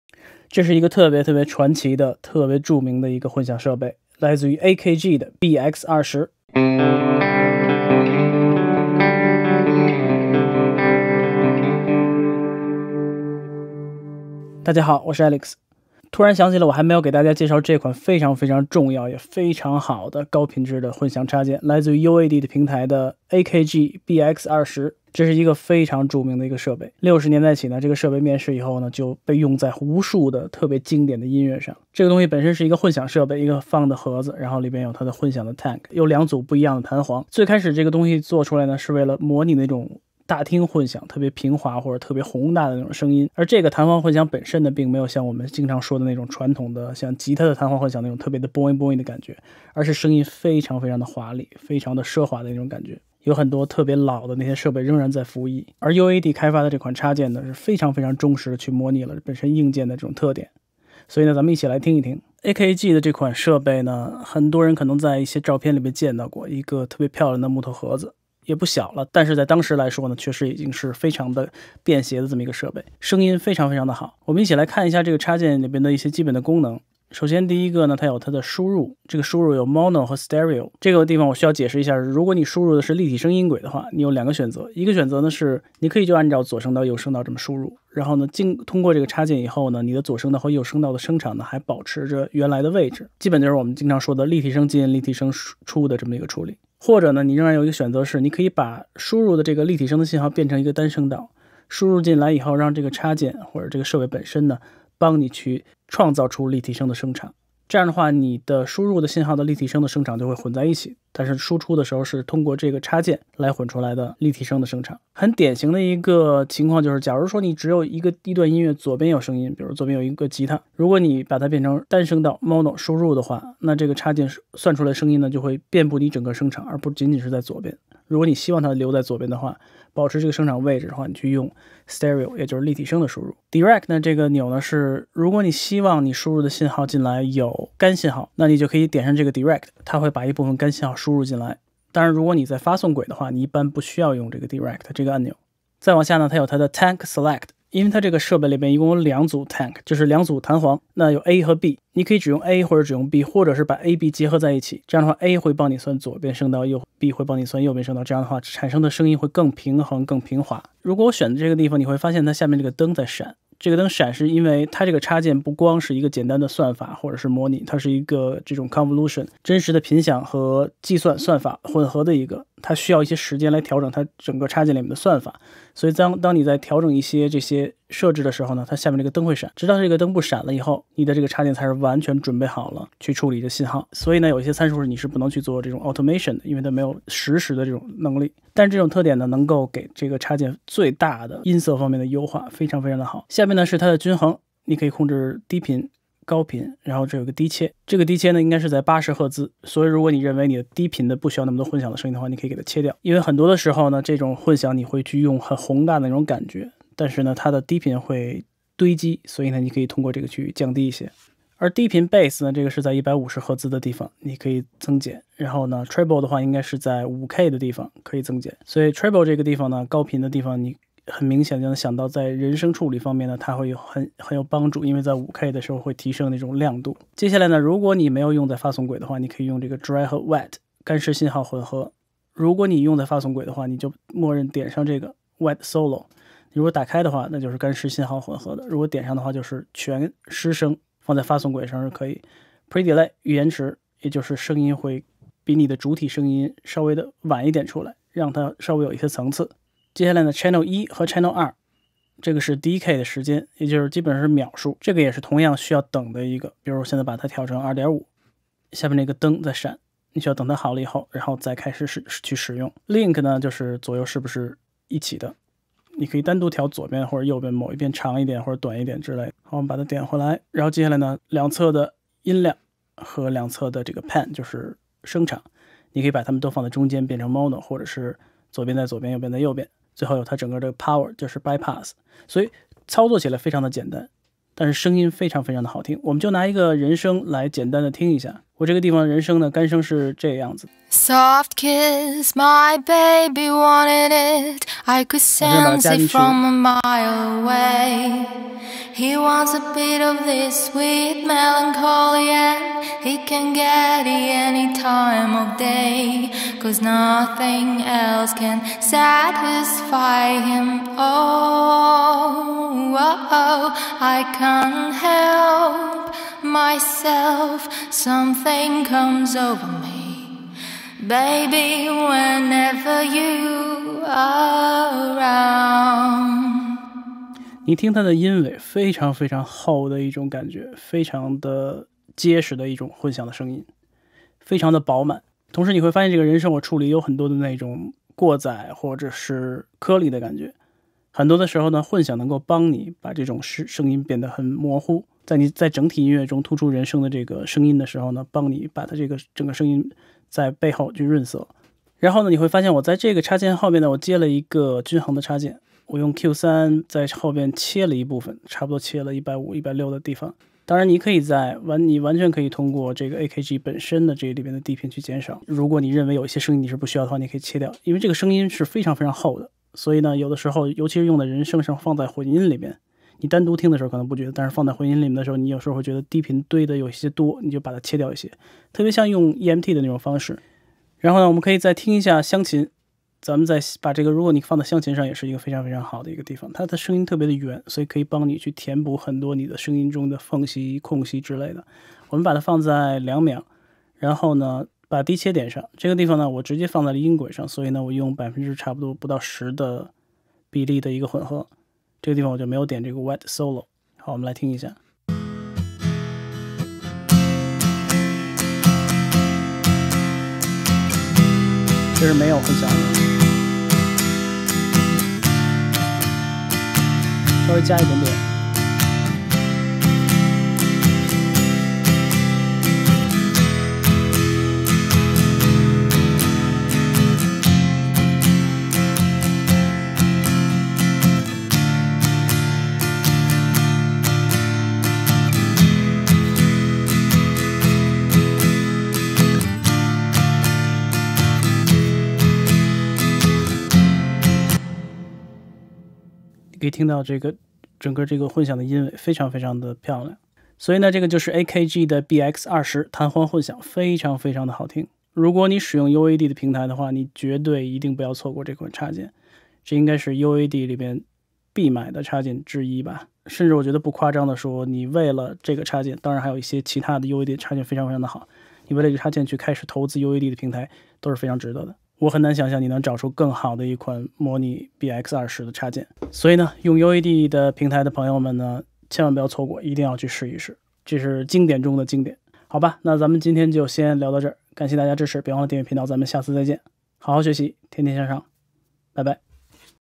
UAD AKG BX20 弹簧混响插件 - 经典再现顶级音质
混音教学 60年代末期，AKG的BX20弹簧混响已经面试马上成为了很多录音棚工作室的最爱，便携的体积和无与伦比的音色让这款设备成为了这几十年流传下来的经典，作为弹簧混响但是有着非常平滑宏大的音色，UAD的模拟技术更为这个设备的插件版本增加了只有数字领域才可能具备的功能，毫无疑问这可能是UAD迄今推出的最好的混响插件之一，绝对不要错过。